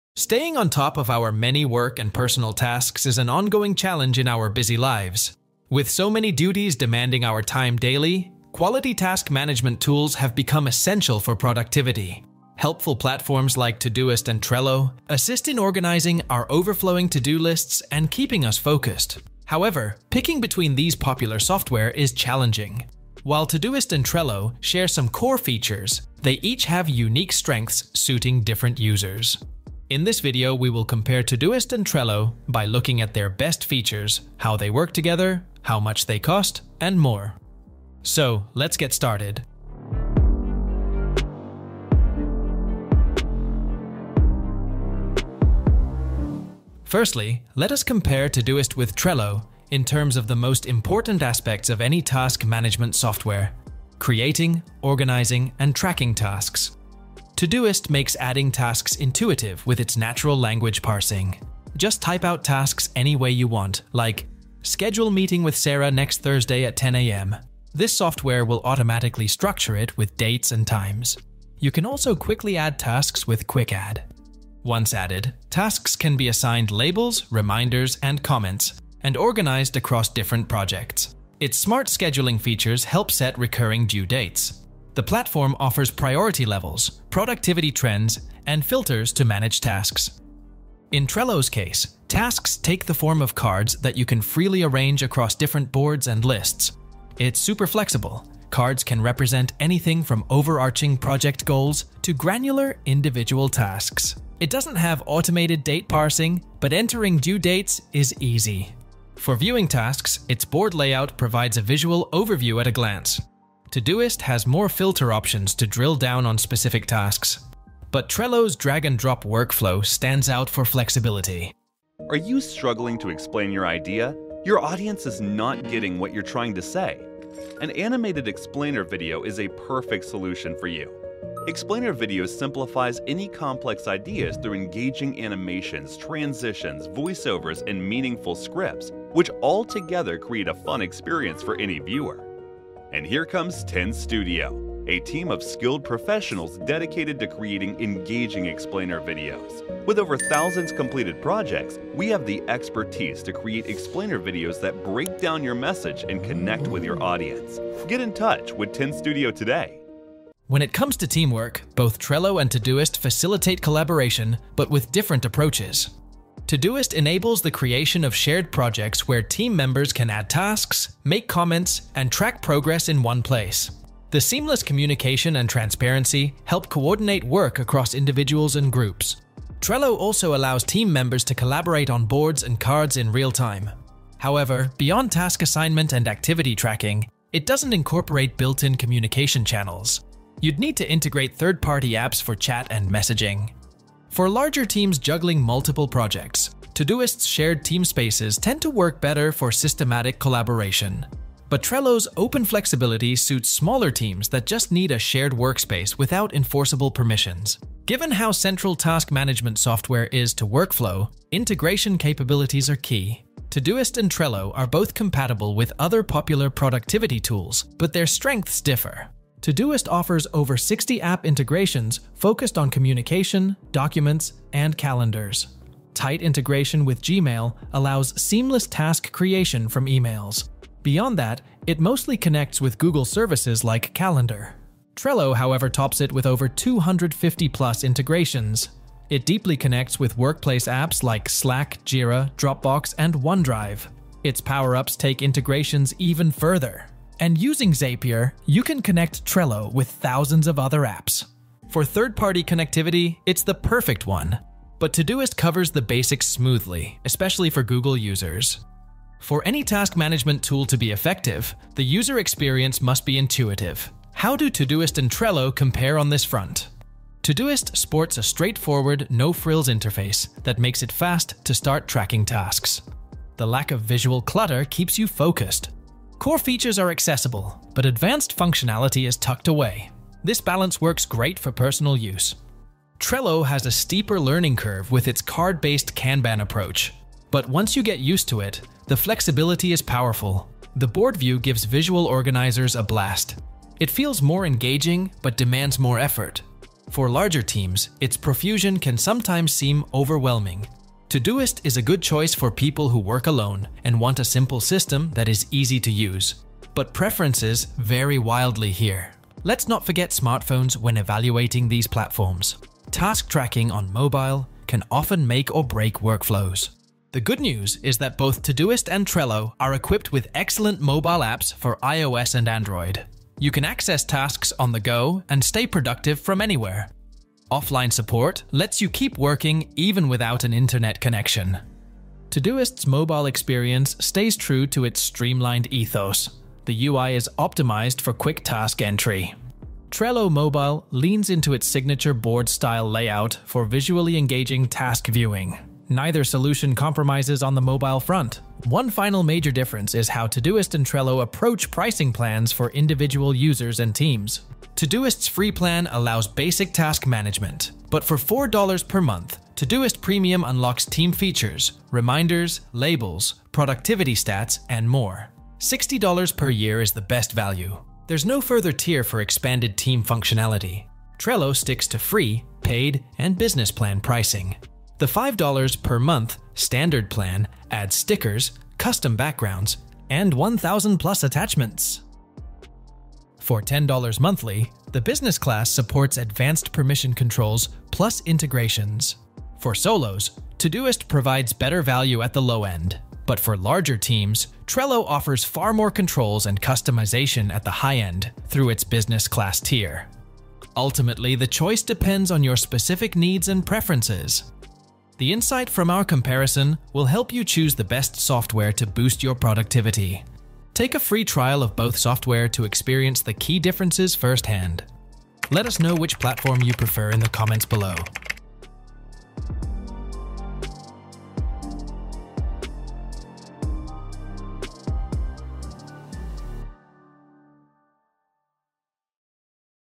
Todoist Vs Trello | Which Task Management Software is Better?
Struggling to find the perfect task management tool to keep your work and personal life organized? In this video, we break down the strengths and weaknesses of two popular platforms: Todoist and Trello. Explore key features such as task creation, organization, collaboration, integrations, ease of use, mobility, and pricing to make an informed decision that suits your unique needs.

Intro and Animation Credit:
10Studio: